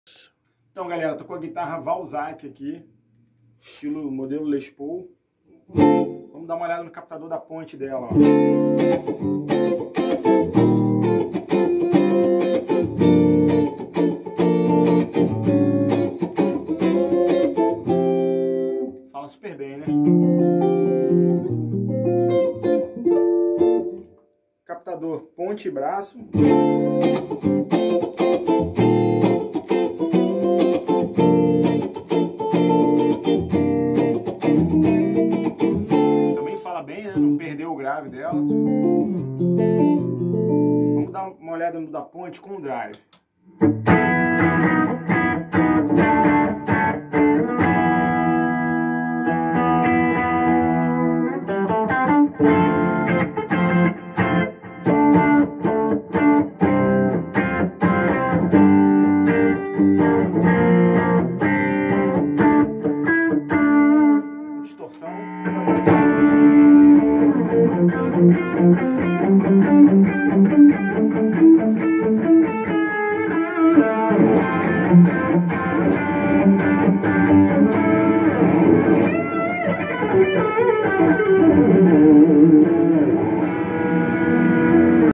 Vídeo Demonstração da Guitarra Walczak a venda no Mercado Livre
Com a participação do Professor André Barros na Escola de Música Fábrica Musical